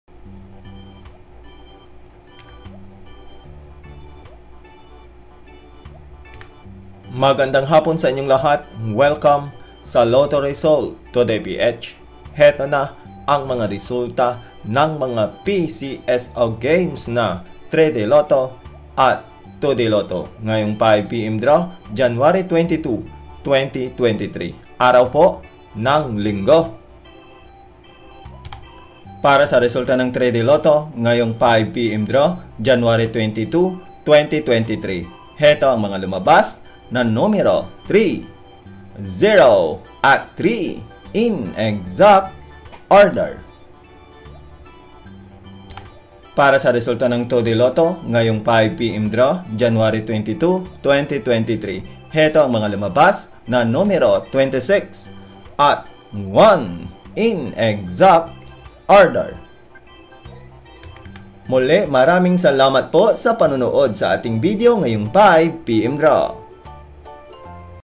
Lotto Result Today 5pm January 22, 2023 Swertres Ez2 Pcso
Lotto result today 5pm Draw January 22, 2023 (Sunday) The 5pm afternoon draw pcso lotto results today are 3D Lotto (Swertres Result Today) and 2D Lotto (Ez2 Result Today).